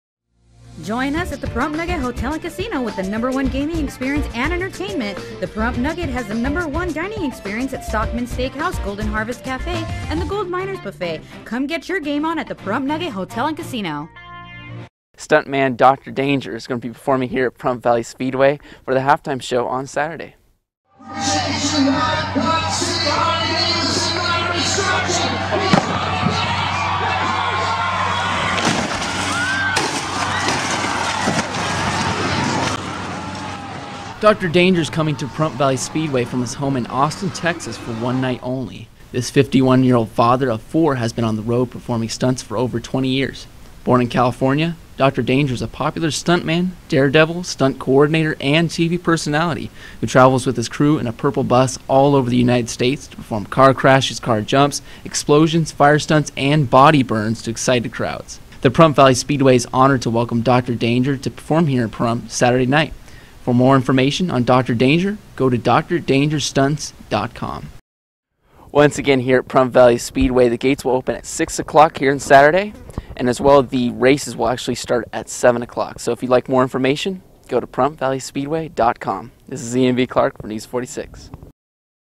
05/08/2013 Dr. Danger Stuntman Pahrump Valley Speedway
If you love the races, dare devil stunts, mixed in with some fire and explosions ... You want to make sure you are at Pahrump Valley Speedway Saturday night for Dr. Danger.